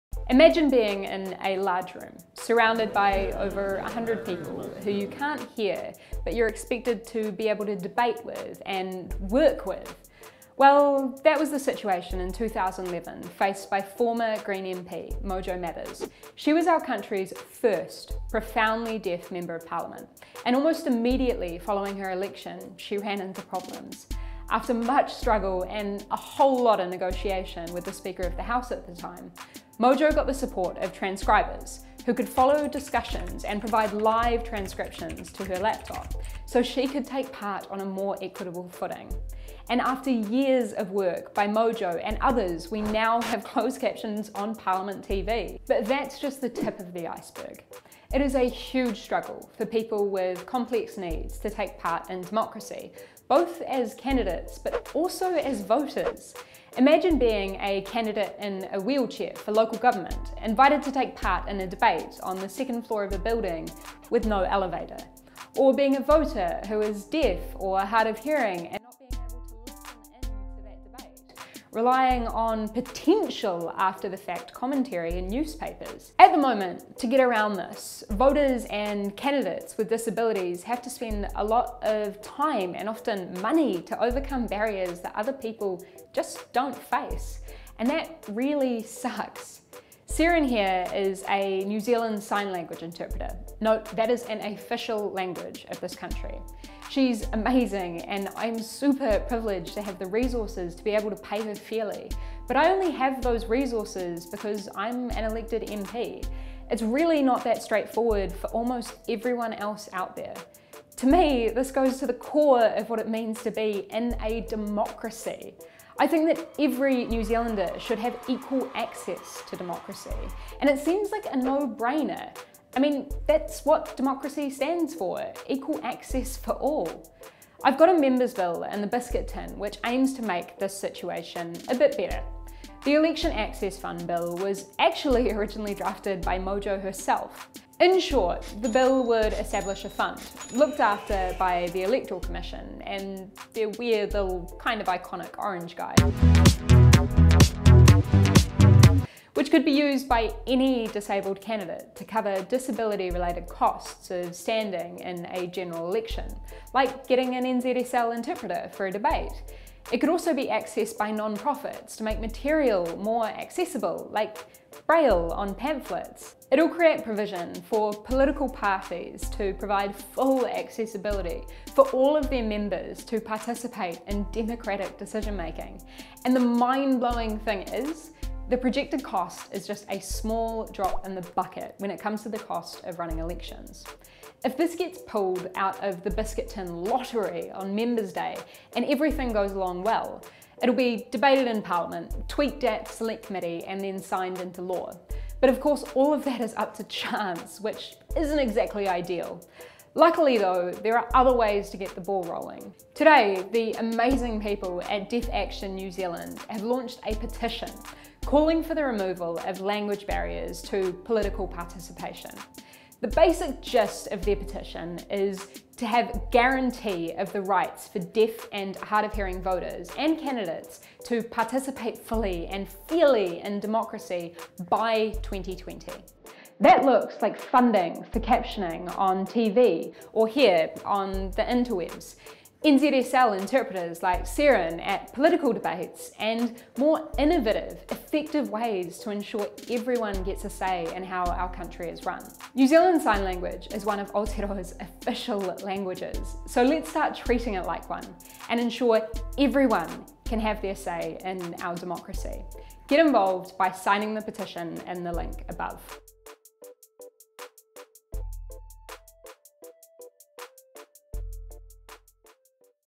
Right to Participate
Article 29 of the United Nations Convention on the Rights of Persons with Disabilities guarantees equal opportunity to participate in public and political life. That's not currently happening. Here's how you can help